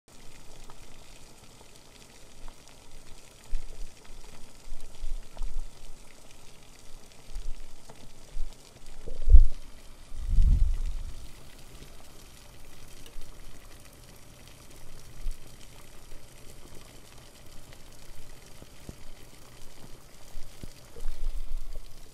Cooking: Boiling Water With Lid On - Sound Effect - FREE
Sound Effect Category:
Cooking

Sound Effect Description:
This is a big pot of boiling corn with the lid on. It is a rolling boil in the pot.